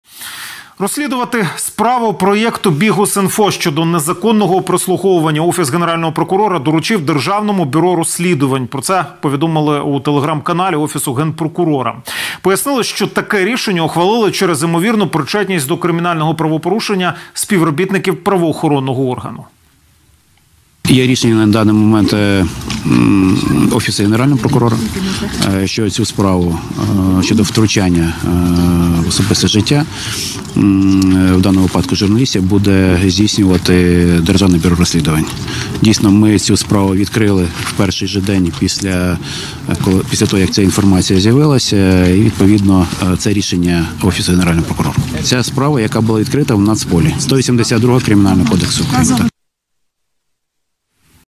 Справа про прослуховування журналістів BIHUS.INFO
В сюжеті телемарафону поінформували, що розслідування справи про прослуховування журналістів BIHUS.INFO доручили ДБР та оприлюднили коментар Міністра внутрішніх справ України Ігоря Клименка з цього приводу